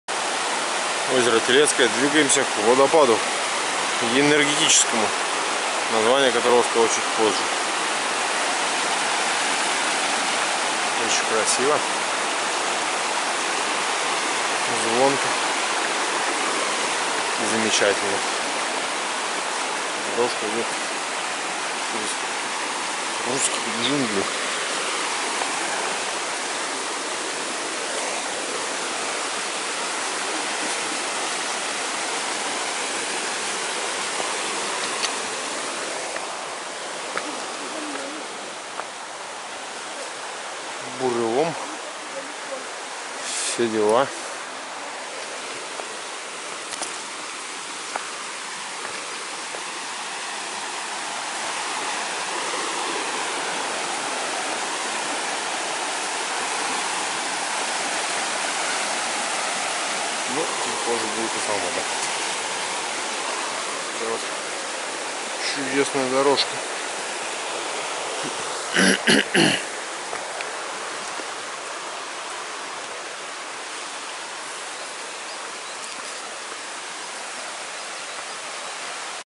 Телецкое водопад рядом с Золотым Озером / Teletskoye waterfall
Project "Life-Travels"
Телецкое водопад рядом с Золотым Озером. Телецкое озеро, водопад рядом с Золотым Озером. До этого места можно добраться только на катере, что мы собственно говоря и сделали.
Teletskoye waterfall near Golden Lake 2. Teletskoye lake, waterfall near Golden Lake. Up to this point can only be reached by boat, what we actually did.